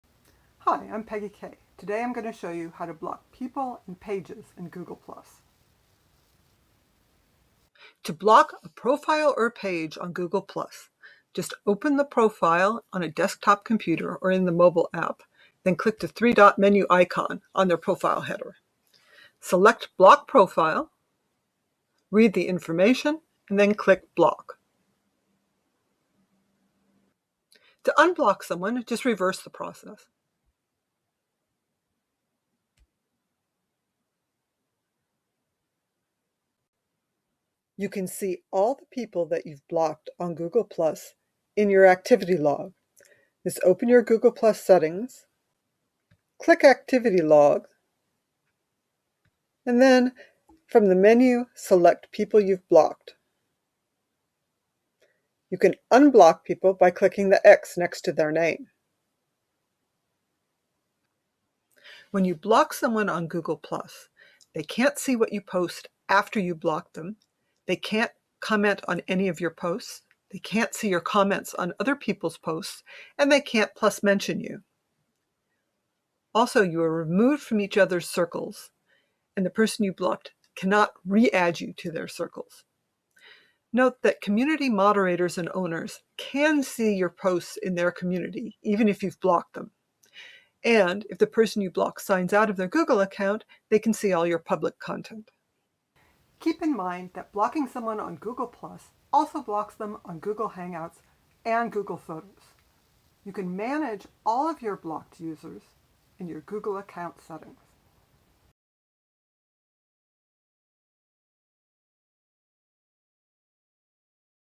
How to block someone on Google+
How to block a person or page on Google+
1. Open their profile on Google+
2. Click the 3 dot menu icon on their profile header
3. Select Block profile
4. Read the information that appears and click Block again

To unblock someone, just reverse the process.

View all the people and pages you've blocked in your Google+ Activity Log: